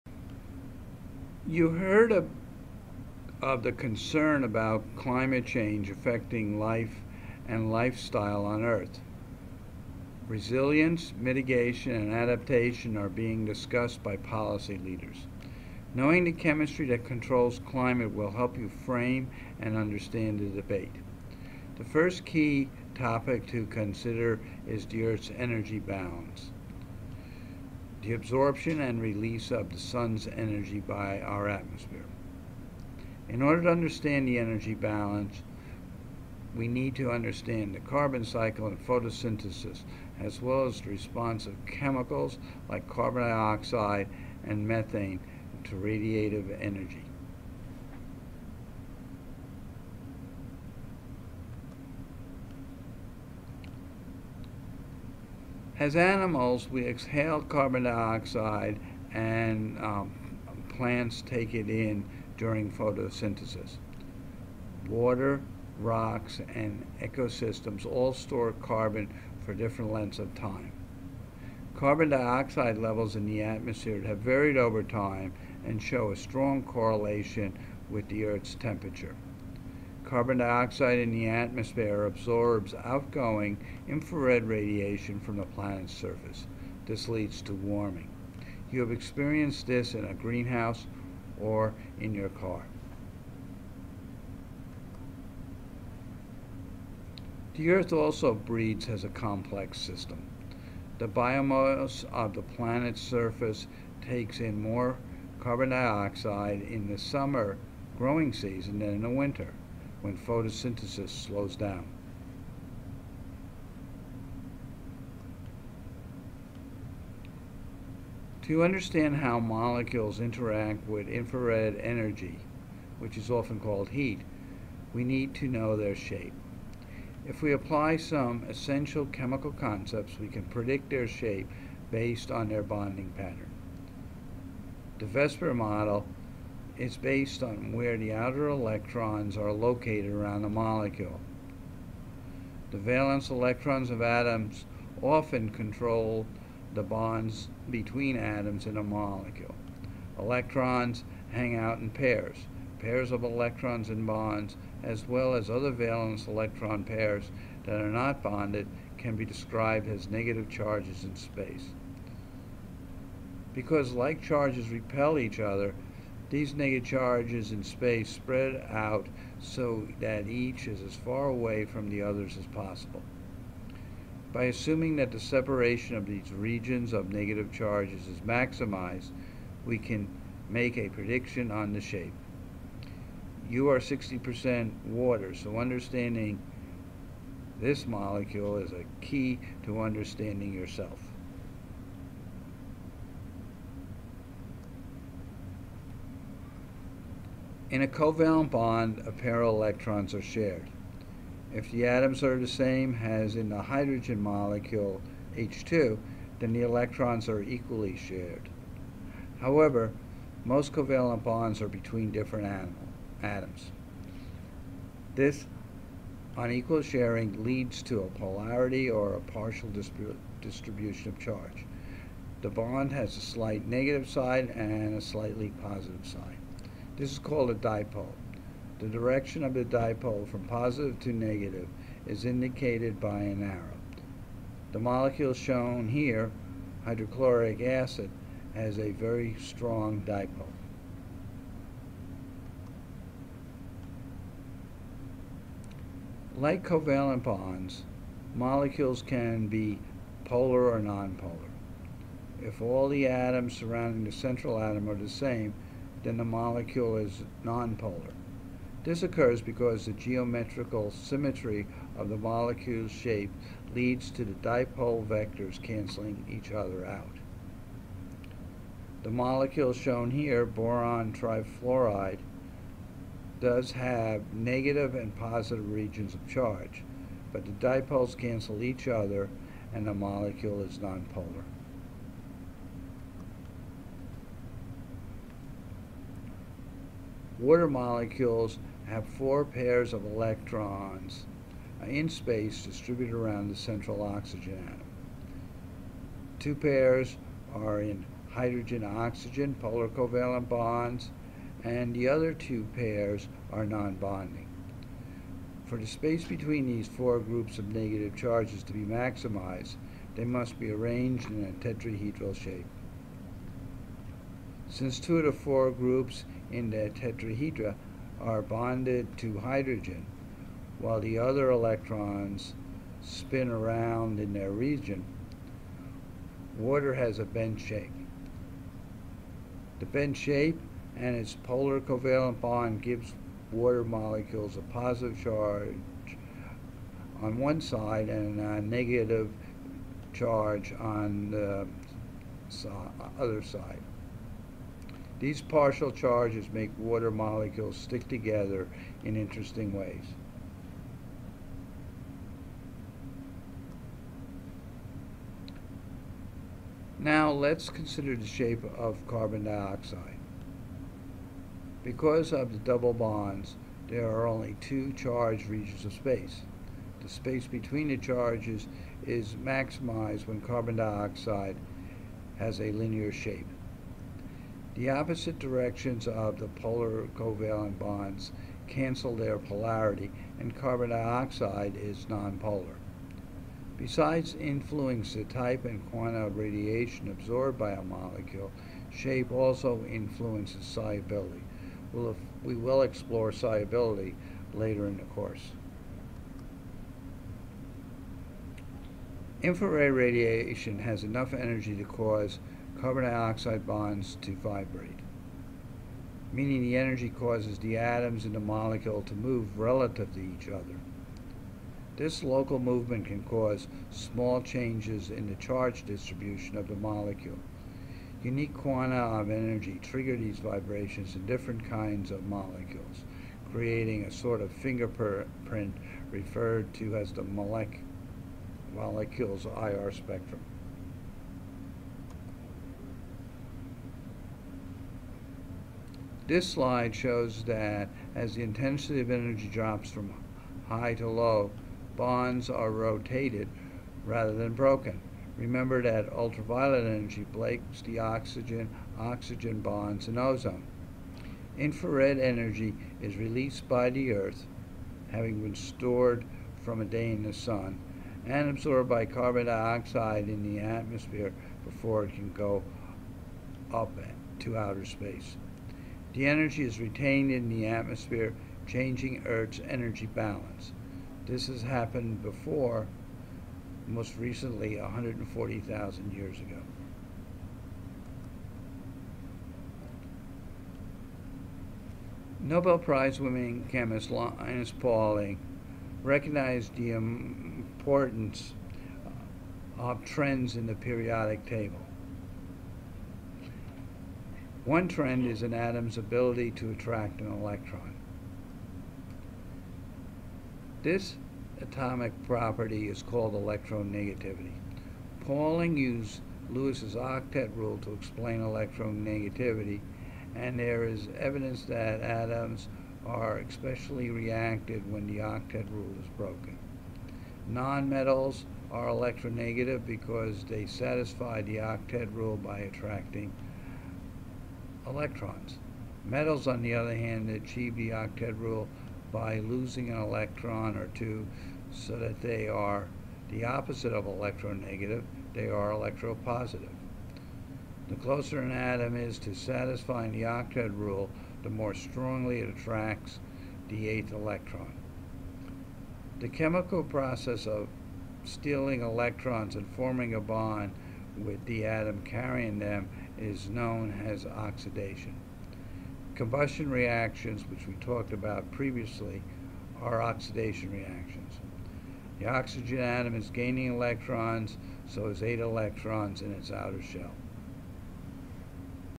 Module 6 - Shape and IR
Lecture on energy balance, greehhouse effect and IR